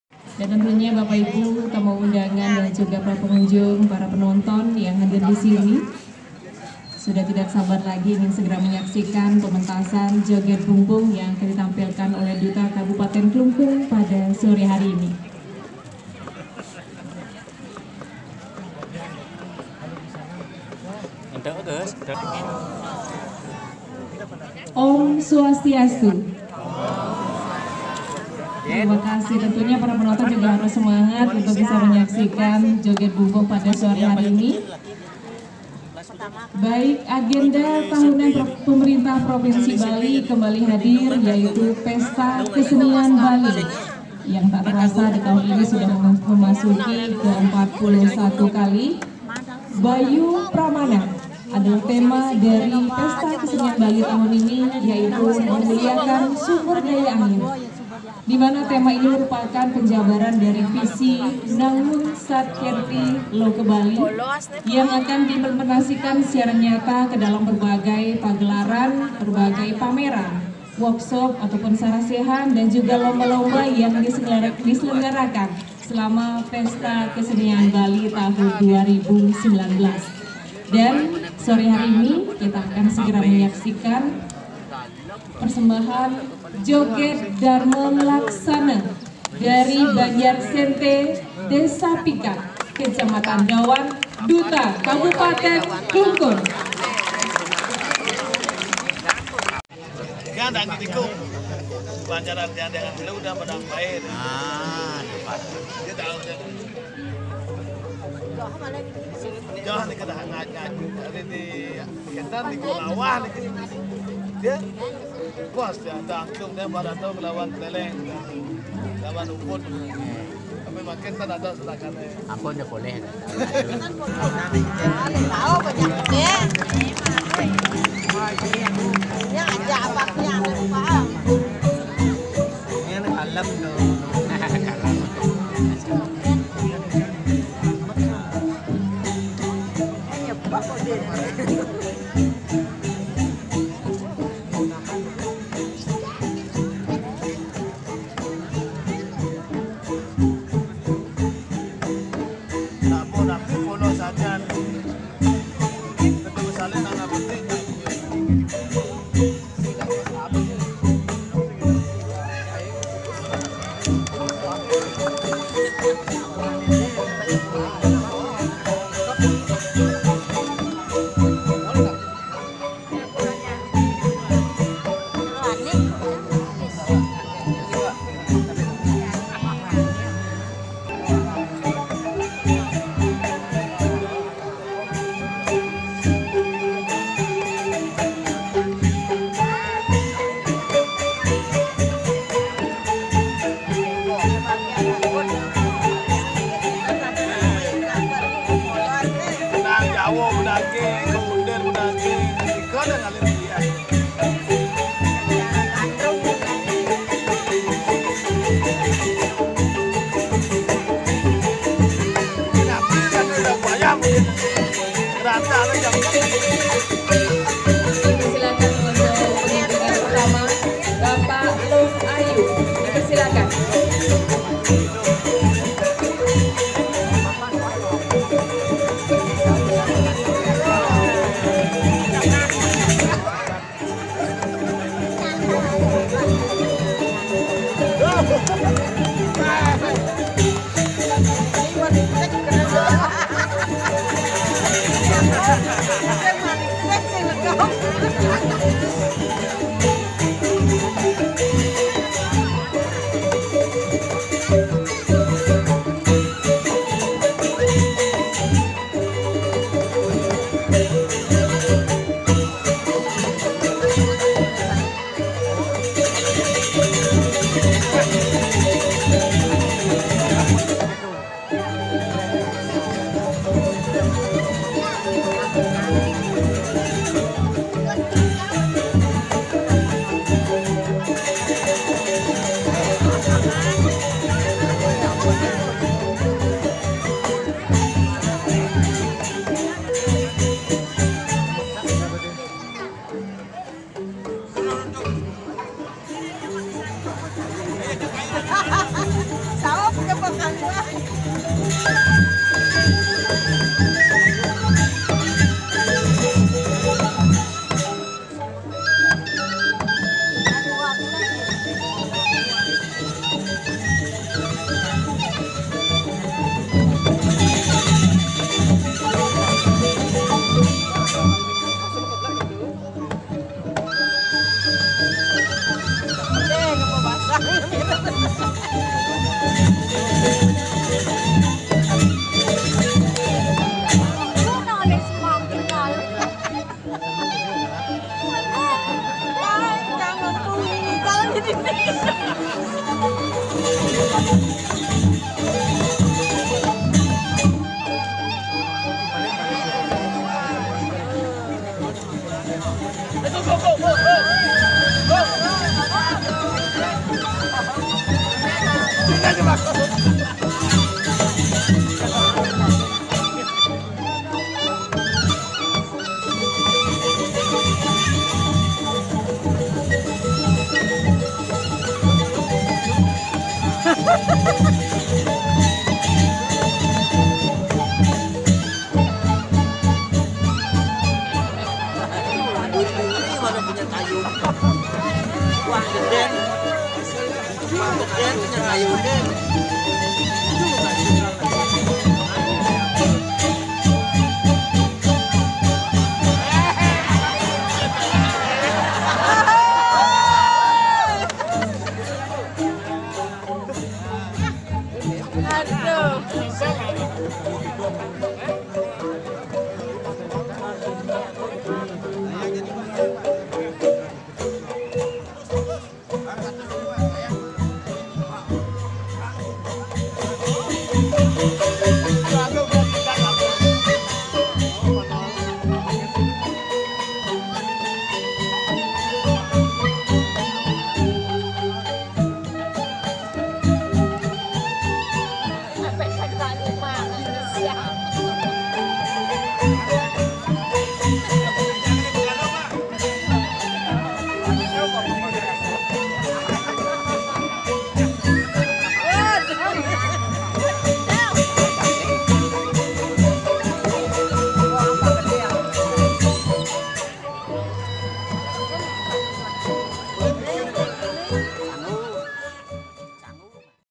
JOGED BUMBUNG DUTA KABUPATEN KLUNGKUNG 2019|| ART CENTER BALI
JOGED BUMBUNG, SEKA JOGED DHARMA LAKSANA, BR.SENTE,DESA PIKAT, KEC. DAWAN, DUTA KABUPATEN KLUNGKUNG
VIDEO PART 2 JOGED BUMBUNG :

SELAMAT DATANG DI CHANNEL GAME, TOTURIAL, DJ REMIX, EKSPERIMEN,KARTUN
PRANK,, DAN VIDEO MENARIK LAINNYA